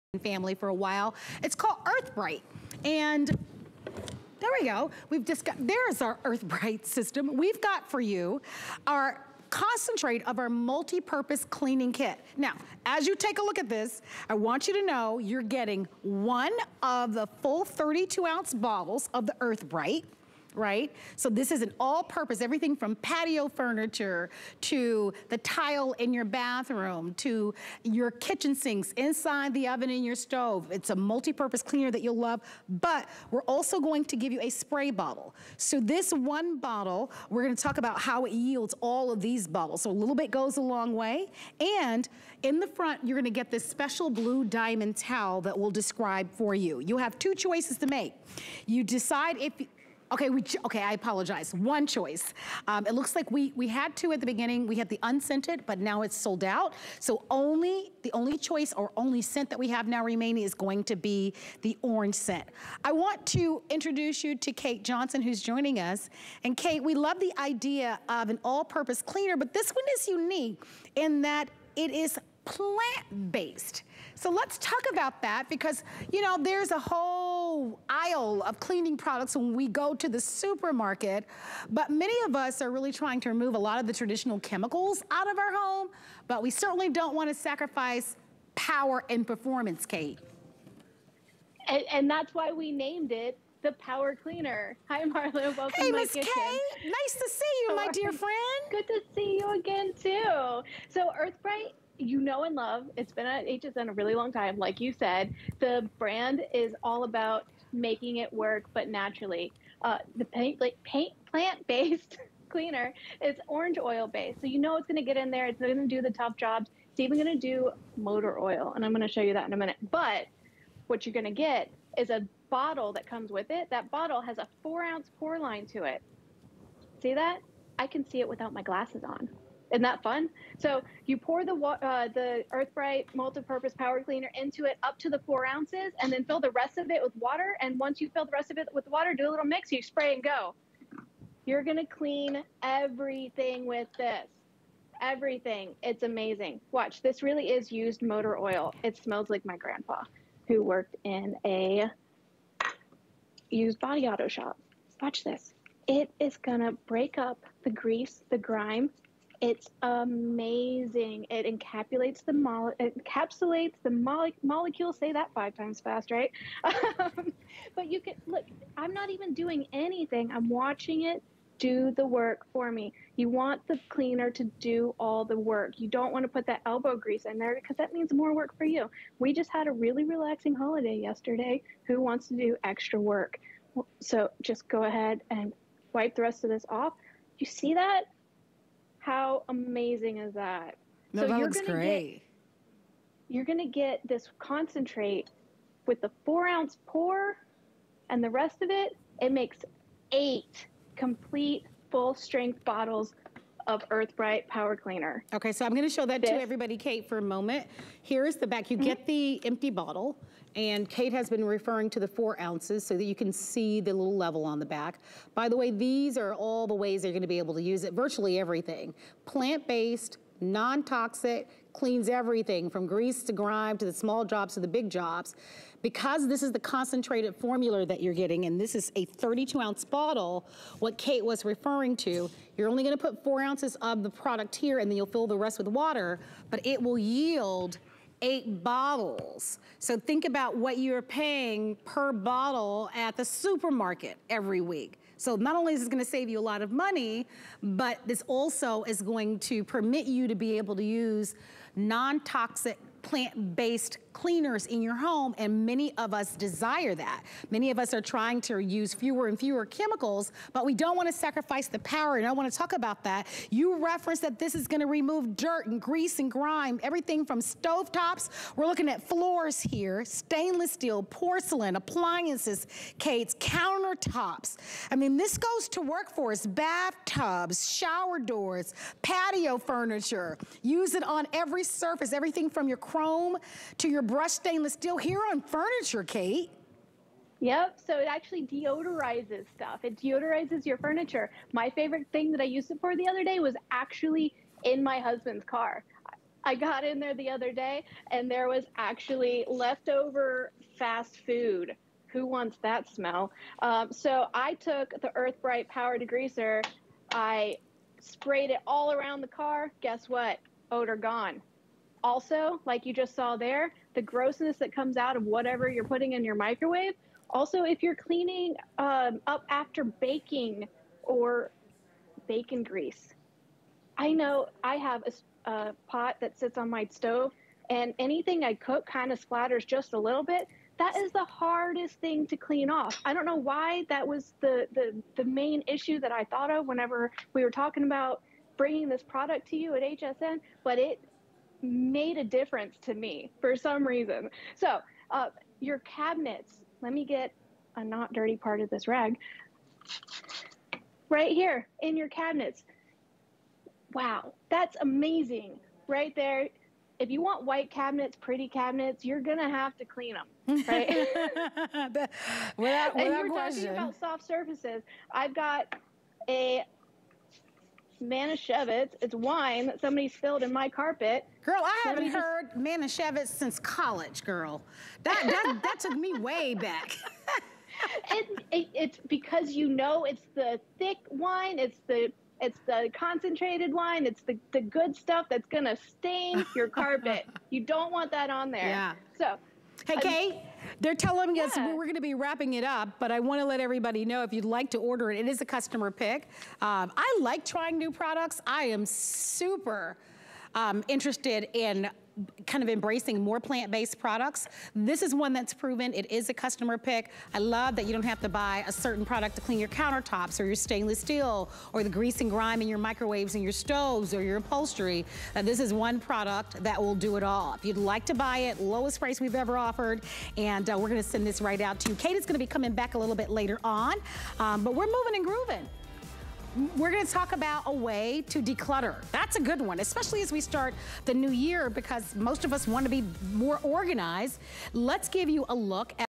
Earth Brite 32 oz. Concentrate Multipurpose Cleaning Kit...
Earth Brite 32 oz. Concentrate Multipurpose Cleaning Kit w/Accessories
 Earth Brite's plantbased ingredients cut through dirt, grease and grime allaround your home. Use it on stovetops, sinks (porcelain   stainless steel), appliances, countertops, cabinets, PVC patio furniture, floor, tile and more. It's formulated so you have the added peace of mind of knowing you are not exposing your family or pets to harmful chemicals, residues or fumes.

    32 oz. bottle of Earth Brite AllPurpose Cleaner

    22 oz. empty plastic spray bottle

    Blue diamond towel